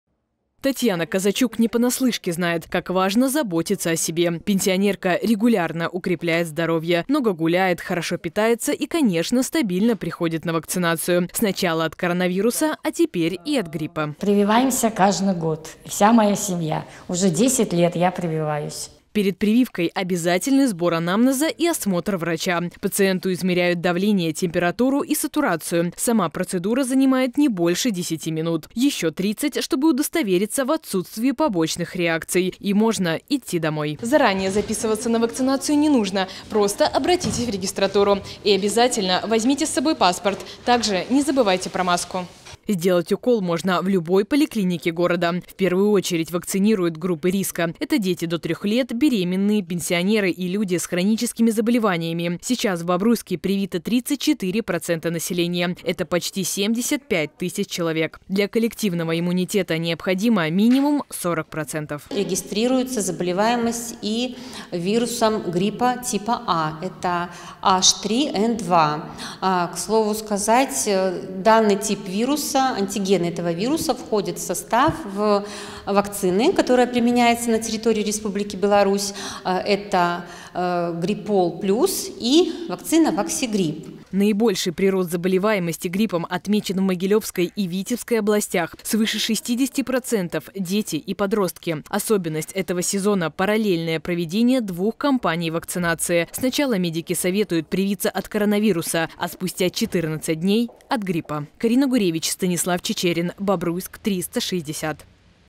Грипп: рост заболеваемости
Проходите на прививку. В Беларуси растет заболеваемость гриппом. Помимо столицы в антирейтинге Могилевская и Витебская области. Кампания по вакцинации продлена до 1 января. В Бобруйске привились почти 75 тысяч человек. Процедурный кабинет работает и в 6 поликлинике.